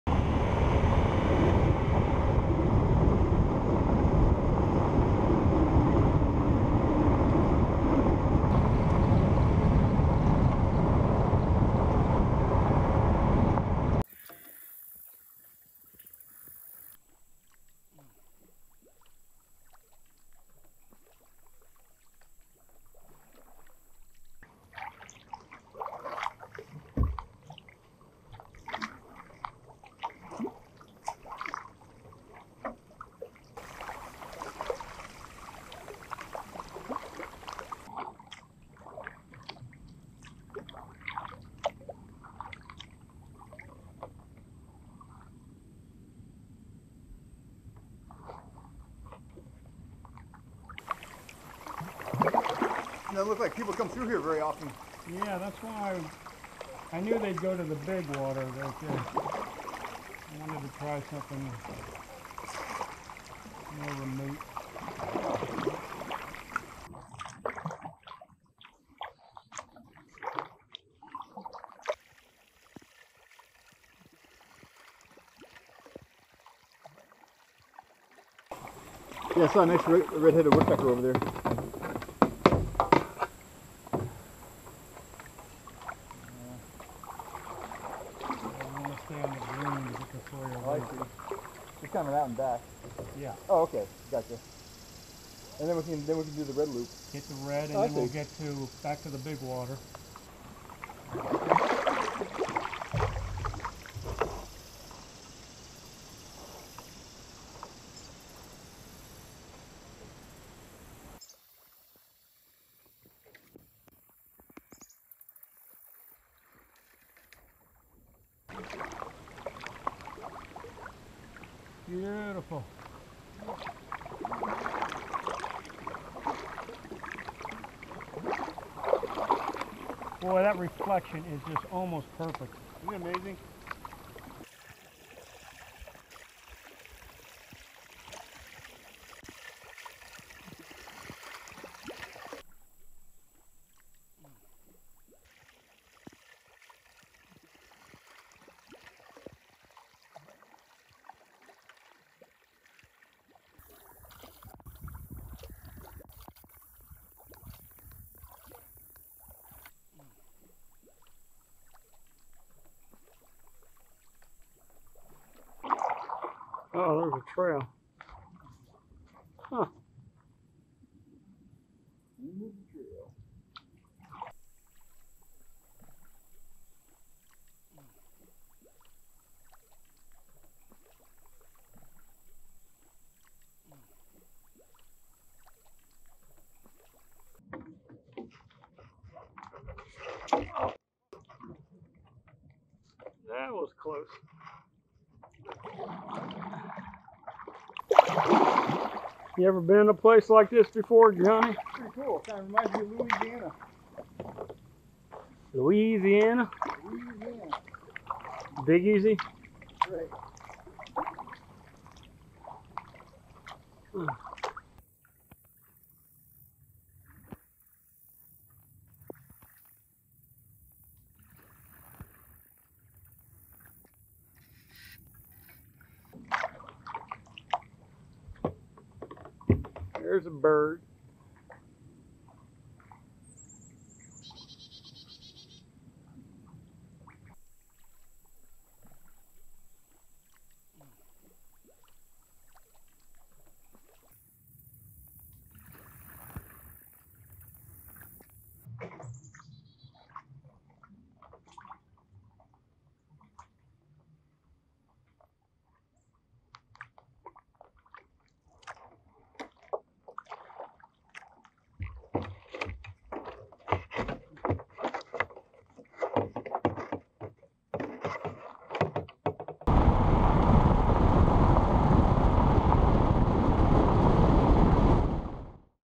Kayaking Alligator River NWR
While enjoying a week on the Outer Banks with friends, John Cushing and I set out to kayak the Alligator River National Wildlife Refuse.